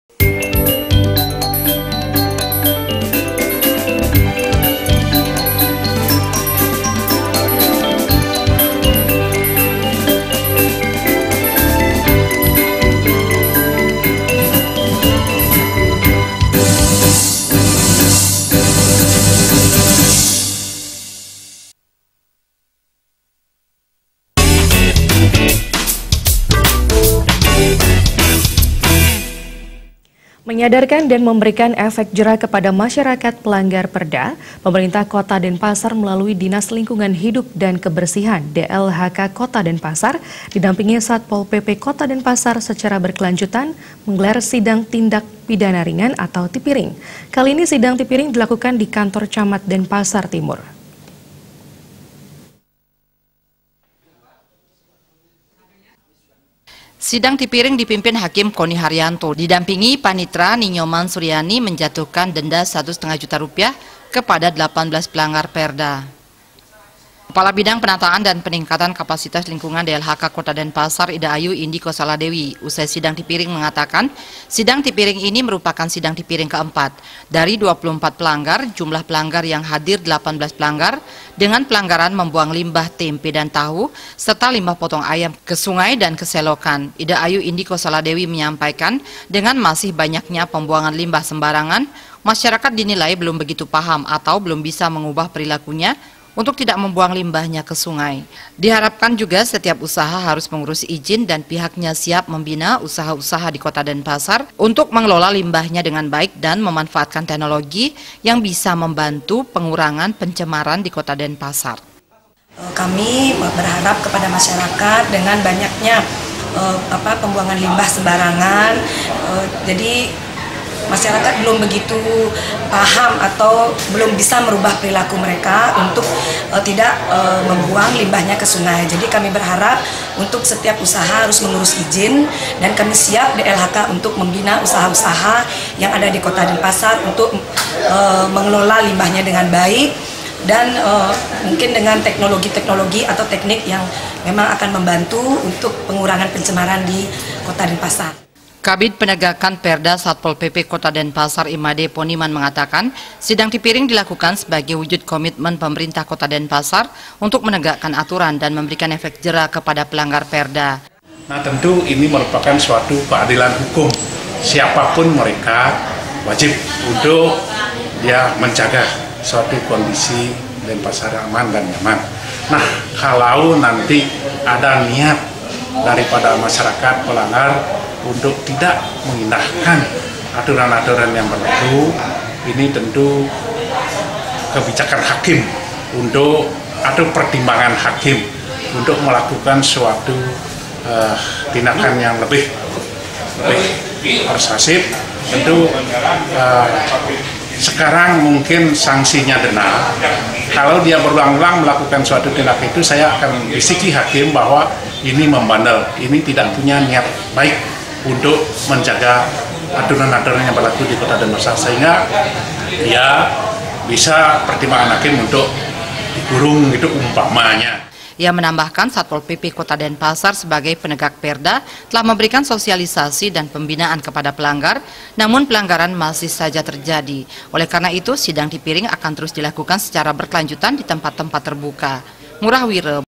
18 PEMBUANG LIMBAH KE SUNGAI JALANI SIDANG TIPIRING
MENYADARKAN DAN MEMBERIKAN EFEK JERA KEPADA MASYARAKAT PELANGGAR PERDA, PEMERINTAH KOTA DENPASAR MELALUI DINAS LINGKUNGAN HIDUP DAN KEBERSIHAN, DLHK, KOTA DENPASAR, DIDAMPINGI SATPOL PP KOTA DENPASAR SECARA BERKELANJUTAN MENGGELAR SIDANG TINDAK PIDANA RINGAN, TIPIRING. KALI INI SIDANG TIPIRING DILAKUKAN DI KANTOR CAMAT DENPASAR TIMUR.